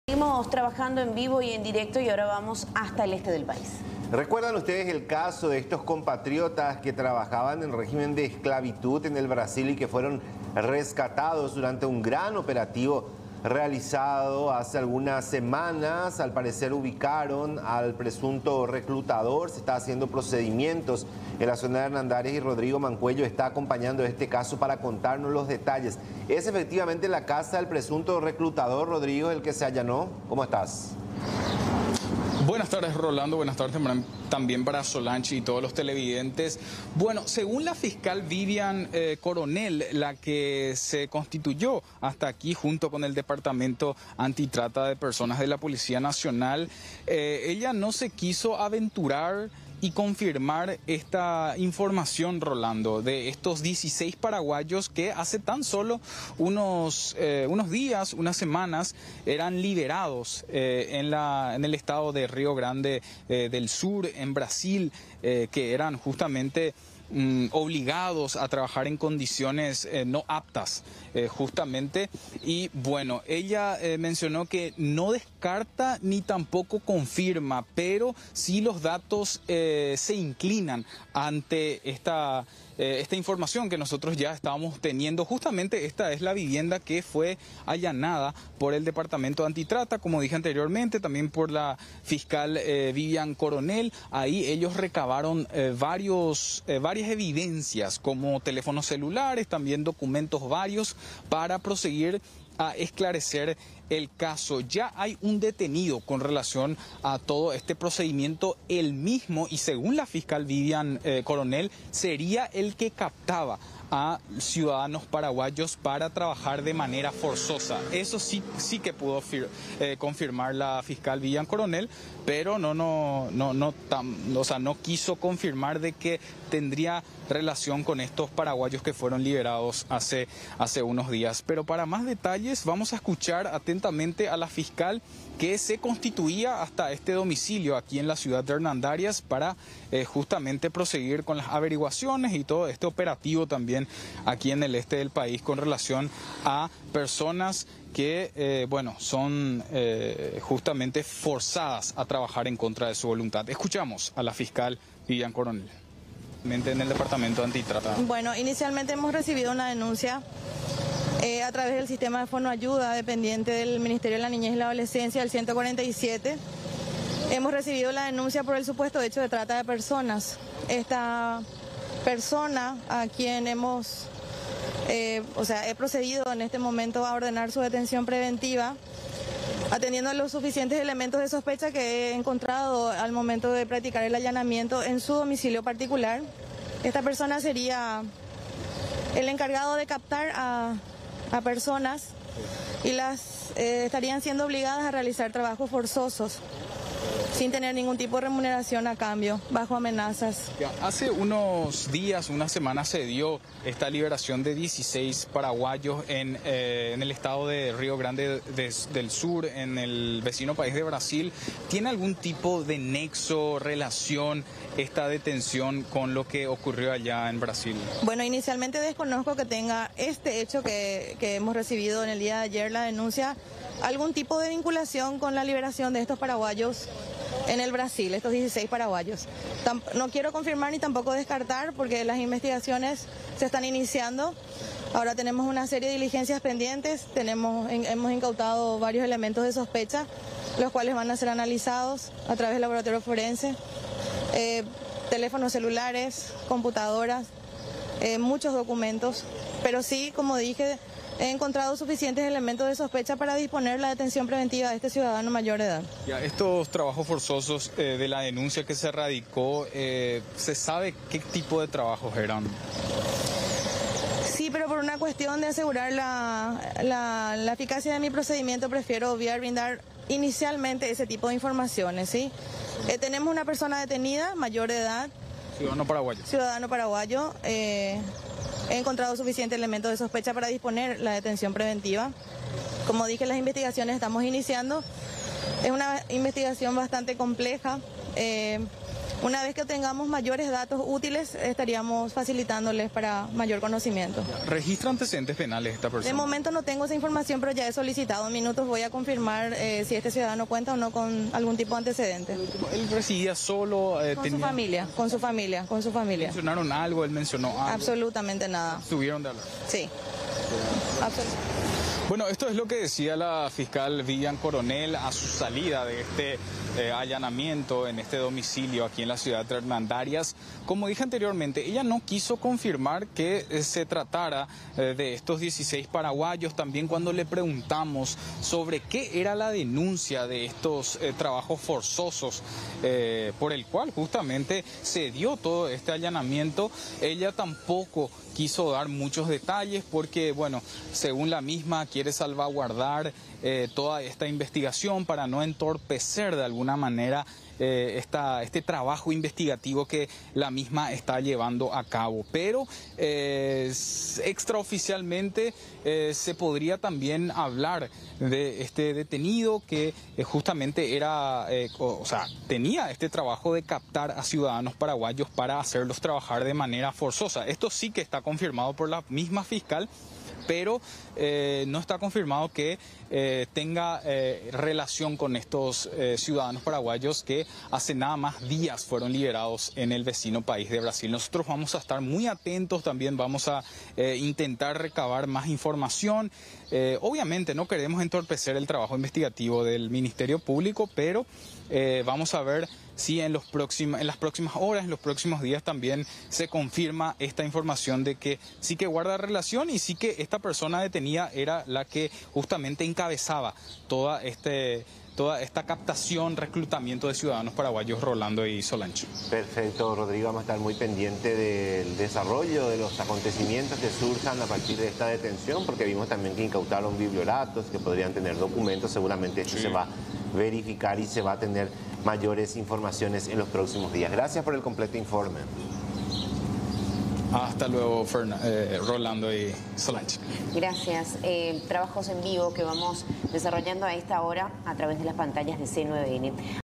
Paraguayos esclavizados: Hay un detenido
📍 El allanamiento guardaría relación con los paraguayos liberados en el vecino país. El detenido sería el "reclutador" que los llevó a Brasil. 👇